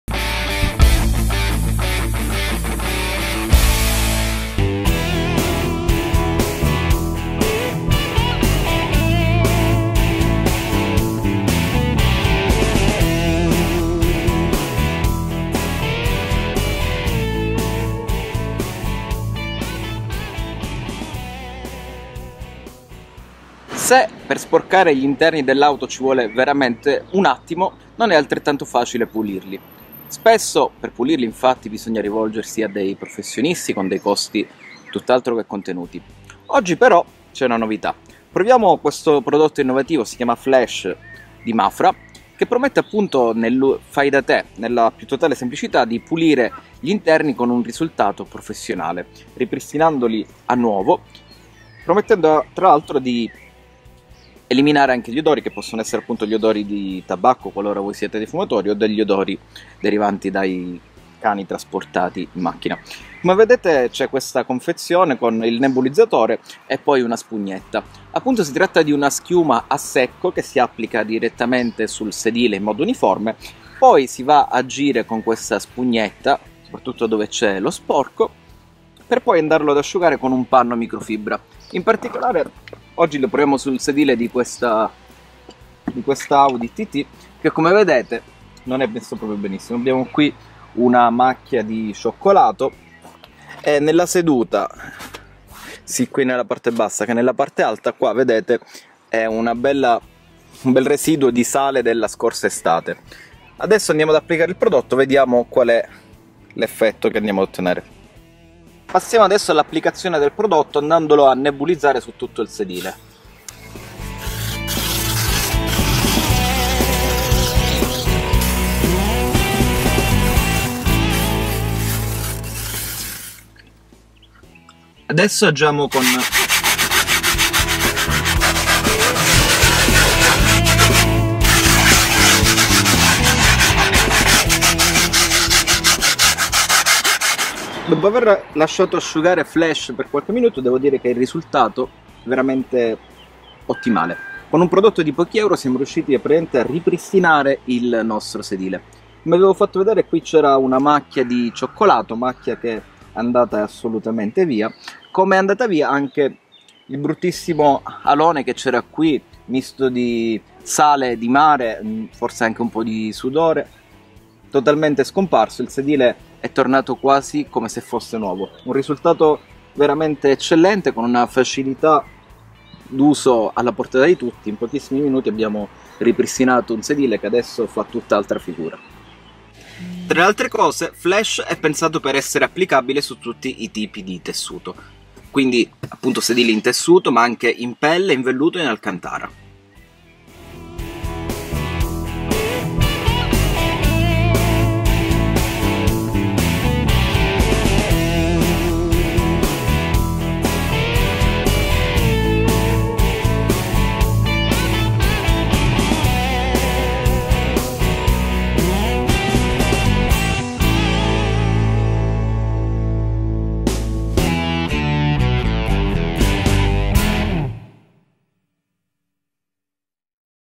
MA-FRA FLASH | Test di pulizia dei sedili
Una prova diretta contro le macchie e i residui rimasti sulle sellerie e all'interno di un'auto, utilizzando un prodotto specifico per la pulizia dell'abitacolo e studiato pensando alle diverse tipologie di rivestimenti.

Servizio: Gianluca Mauriello
Immagini: Giorgia Ceraulo, Gianluca Mauriello
Montaggio: Mariano Tedesco